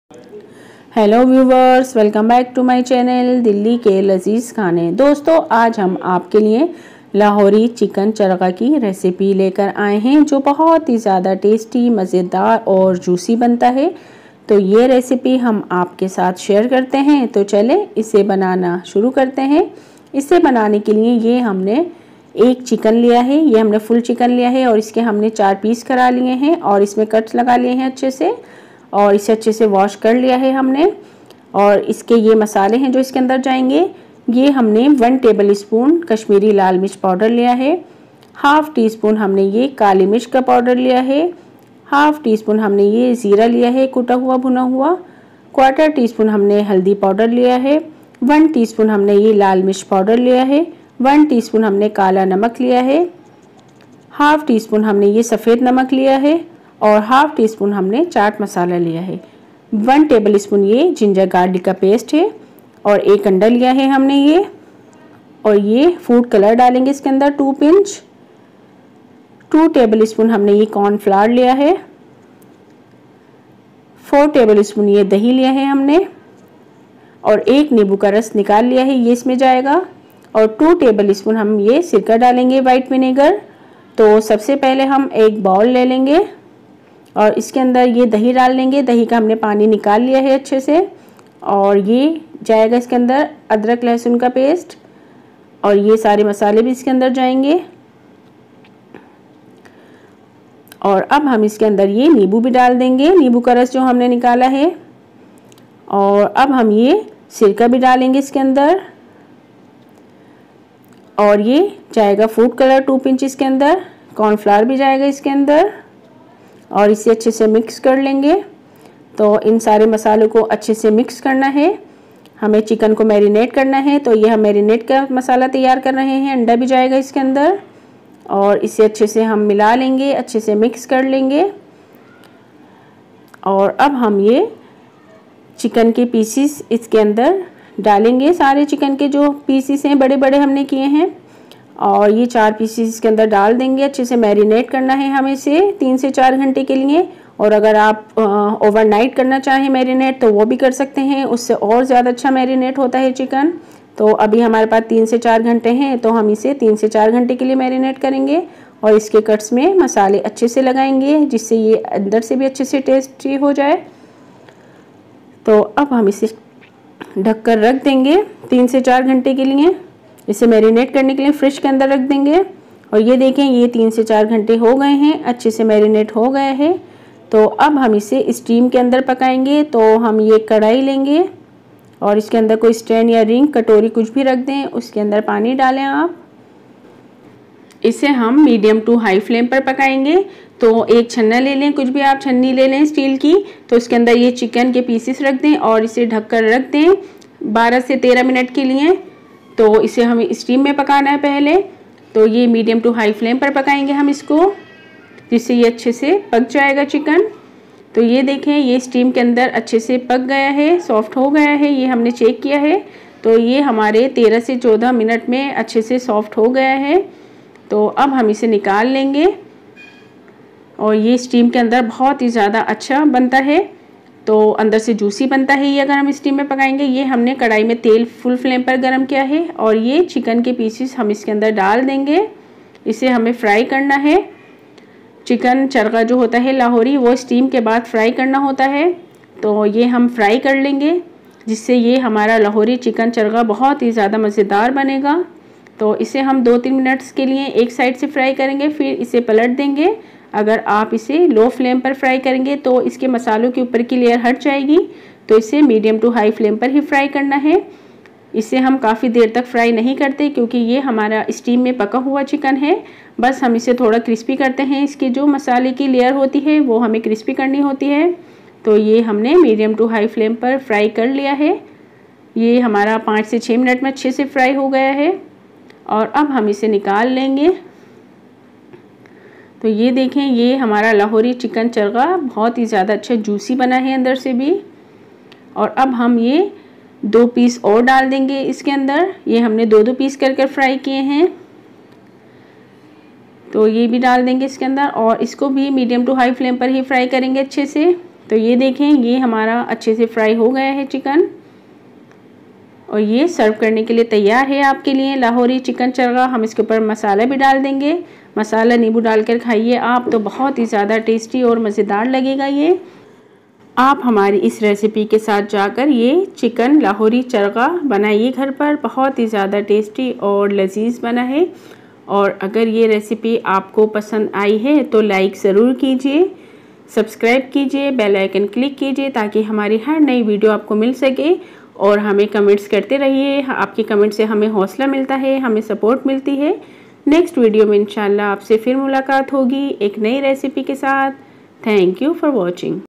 Lahori Chicken Chargha | Steamed And Fried | Recipe By Dilli Ke Lazeez Khane
In this video, I am sharing recipe of Lahori chicken Chargha steamed and fried.